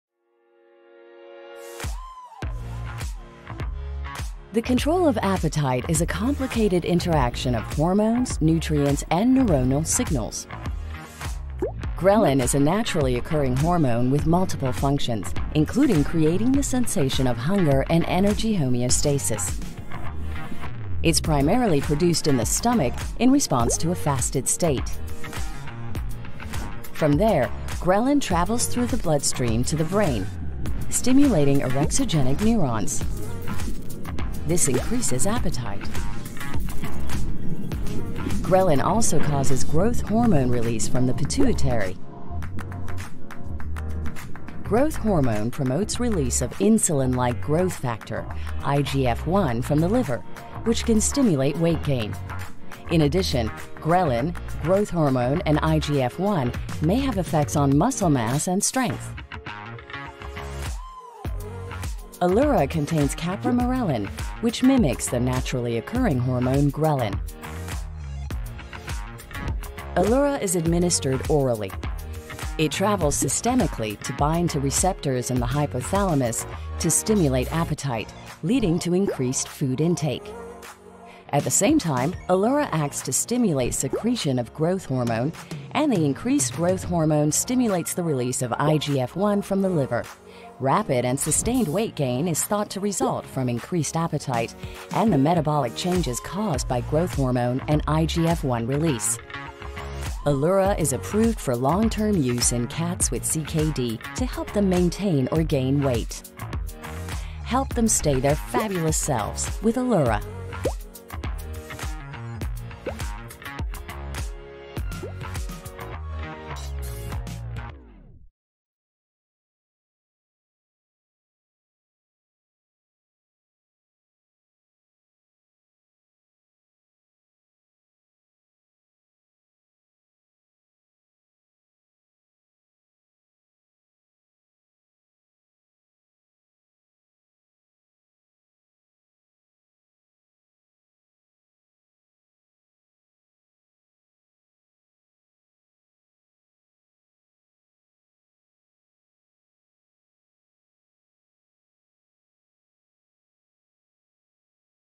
Elura™ (capromorelin oral solution) Mechanism of Action
Learn how Elura mimics the hormone ghrelin.

INDICATION: For management of weight loss in cats with chronic kidney disease.

IMPORTANT SAFETY INFORMATION: For oral use in cats only.  Do not use in cats that have a hypersensitivity to capromorelin, or in cats with hypersomatotropism (acromegaly).  ELURA may increase serum glucose for several hours after dosing; use in cats with current or historical diabetes mellitus has not been evaluated and may not be appropriate.  Use with caution in cats that may have cardiac disease, severe dehydration, or hepatic dysfunction.  ELURA has not been evaluated in cats younger than 5 months of age, or in breeding, pregnant or lactating cats. The most common adverse reactions included vomiting, hypersalivation, inappetence, behavior change and lethargy. for full prescribing Information.  This video is intended for US audience only.